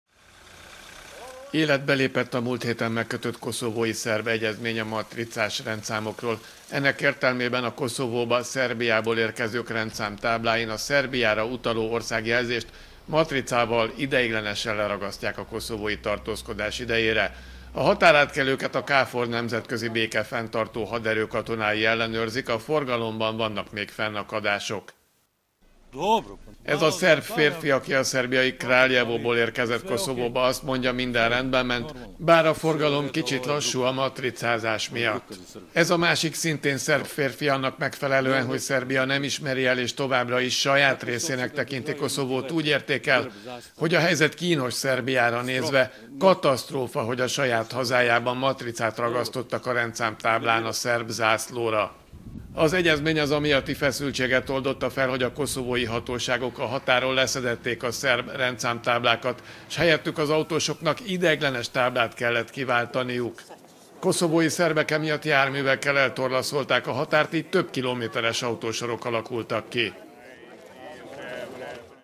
Koszovó: matricával ragasztják le a szerb rendszámtáblákat
A határátkelőkön már alkalmazzák a múlt héten megkötött koszovói-szerb egyezményt.